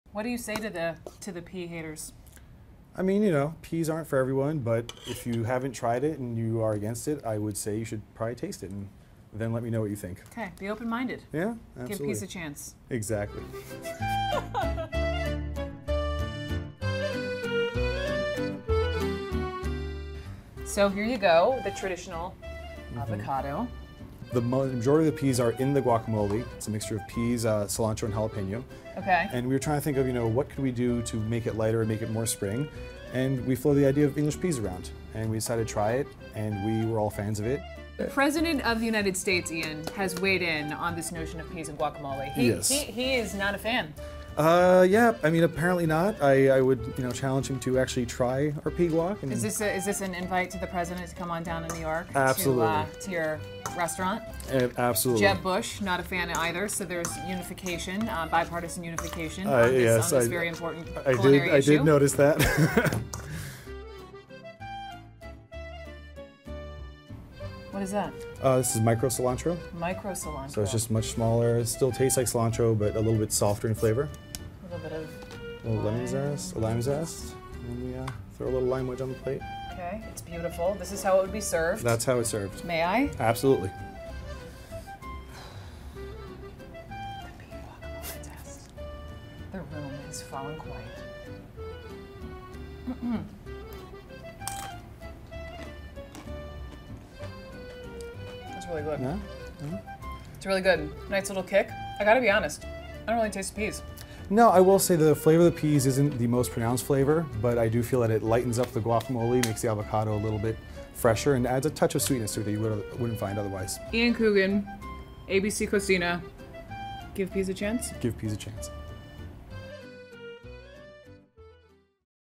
Make peace with putting peas in your guacamole
CNN's Brooke Baldwin meets Ian Coogan, one of the mastermind chefs behind adding peas to your guacamole.